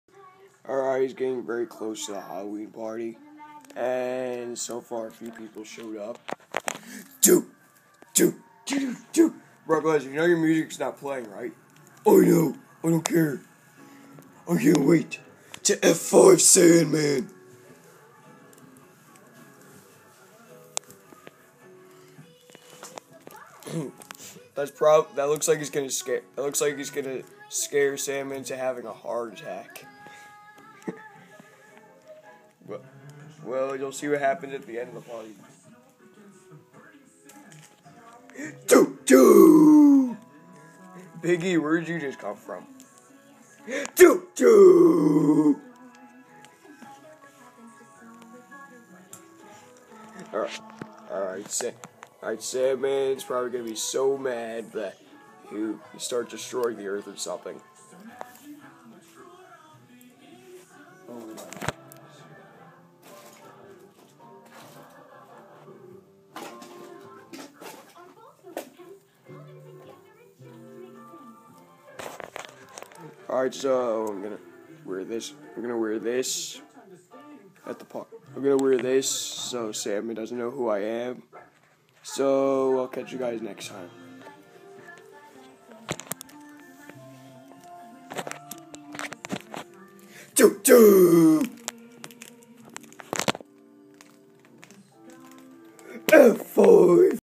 4th Prank on Sandman: Stage 9: The Preparations
People start showing up to the Halloween party.